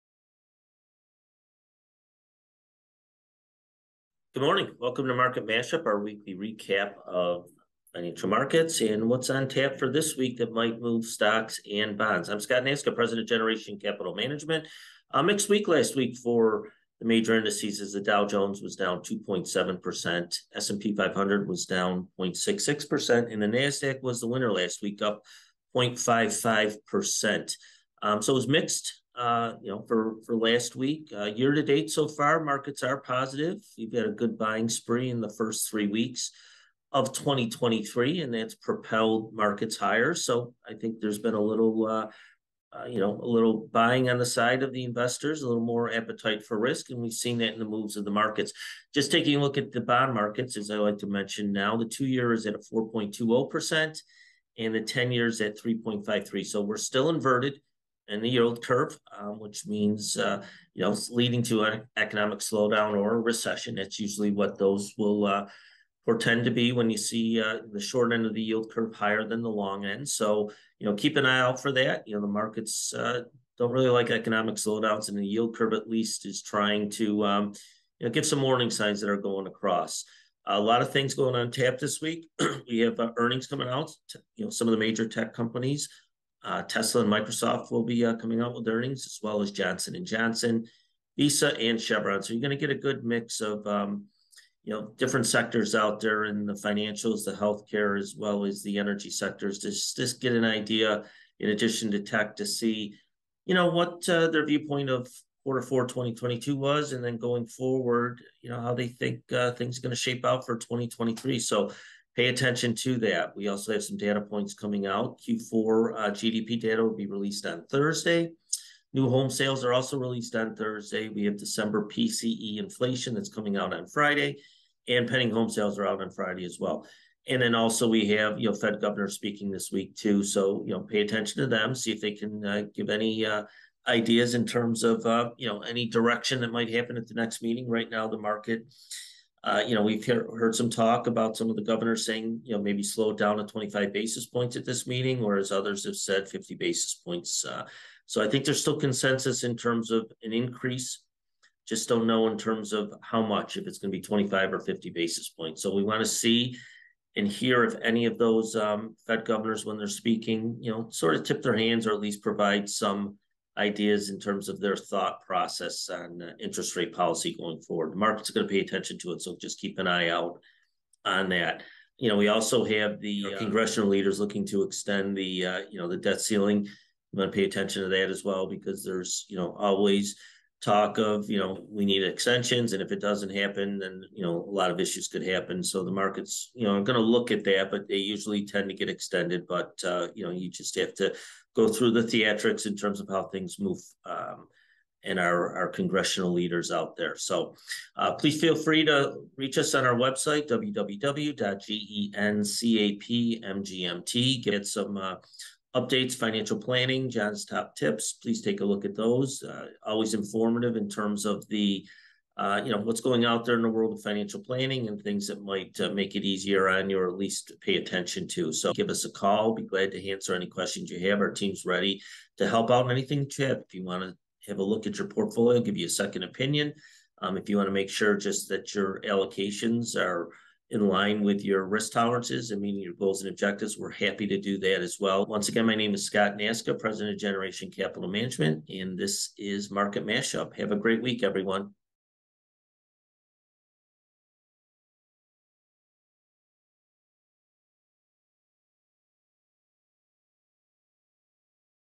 Market Mashup--January 23, 2023
Have a case of the Mondays? A few minutes with GCM President Scott Nasca, CFA as he delivers this week's Market Mashup should be just what the doctor ordered!